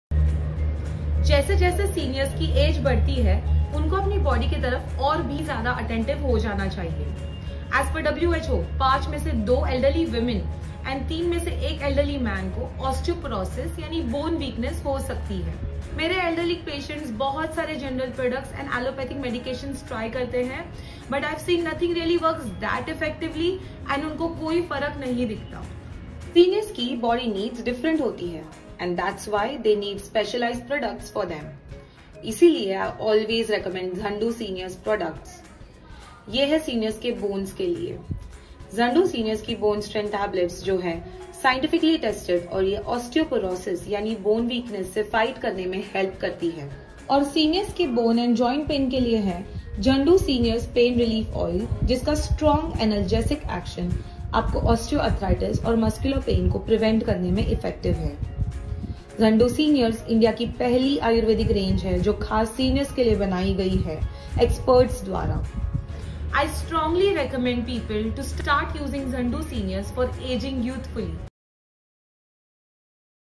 Zandu Seniorz Range
Zandu's Seniorz offers a variety of products to enhance the well-being and comfort of seniors. Explore our range, including pain balms, gels, and bone and joint care solutions. Your loved ones deserve the best, so find the perfect solutions for their needs today! 💪🛒